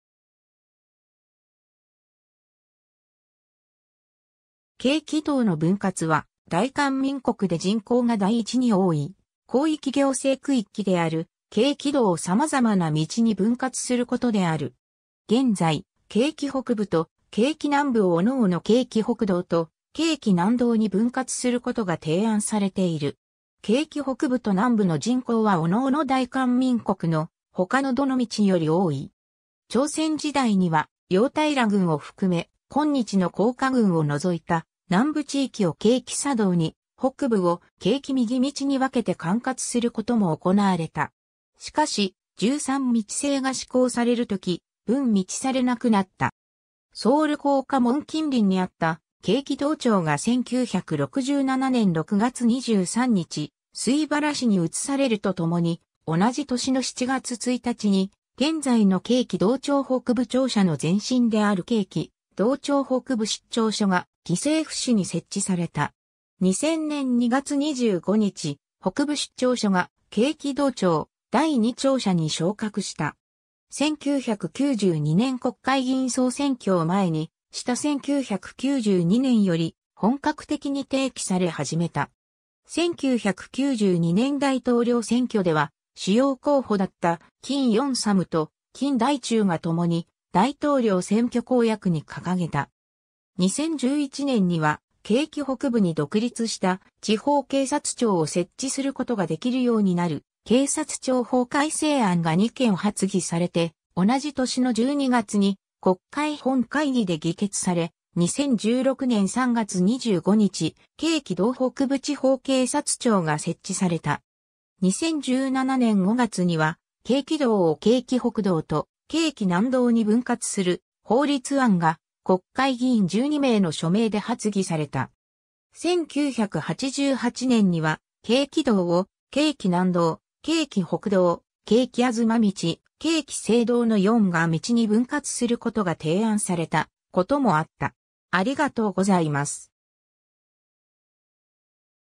京畿道の分割
京畿道の分割（キョンギどう- -ぶんかつ）は、大韓民国で人口が第1に多い広域行政区域である京畿道を様々な道に分割することである。
現在、京畿北部と京畿南部を各々京畿北道と京畿南道に分割することが提案されている。
京畿北部と南部の人口は各々大韓民国の他のどの道より多い。
 朝鮮時代には、楊平郡（正確には楊根郡と砥平郡）を含め、今日の江華郡（喬桐郡含む）を除いた南部地域を京畿左道に、北部（江華郡・喬桐郡含む）を京畿右道に分けて管轄することも行われた。
しかし、十三道制が施行される時、分道されなくなった。
 ソウル光化門近隣（世宗路76-2）にあった京畿道庁が1967年6月23日、水原市に移されると共に、同じ年の7月1日に現在の京畿道庁北部庁舎の前身である京畿道庁北部出張所が議政府市に設置された。
 2000年2月25日、北部出張所が京畿道庁第2庁舎に昇格した。
 1992年国会議員総選挙を前にした1992年より本格的に提起され始めた。
1992年大統領選挙では主要候補だった金泳三と金大中が共に大統領選挙公約に掲げた。
 2011年には、京畿北部に独立した地方警察庁を設置することができるようになる警察庁法改正案が2件発議されて、同じ年の12月に国会本会議で議決され、2016年3月25日、京畿道北部地方警察庁が設置された。
 2017年5月には、京畿道を京畿北道と京畿南道に分割する法律案が国会議員12名の署名で発議された。
 1988年には京畿道を京畿南道・京畿北道・京畿東道・京畿西道の4ヶ道に分割することが提案されたこともあった。